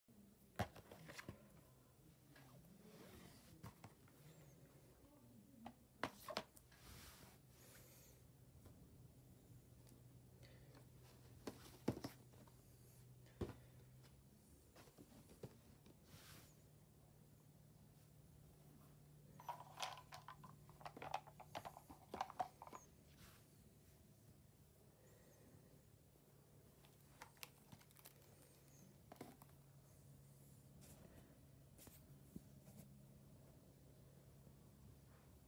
What I got from Torksey car boot sale.
What I got from there,
1. The very best of British steam today dvd.
2. Flying Scotsman DVD
3. The Cat in the hat Funko Pop
4. Fox in Socks Funko Pop (flocked version)
5. Push along Toby
6. Tomica Gordon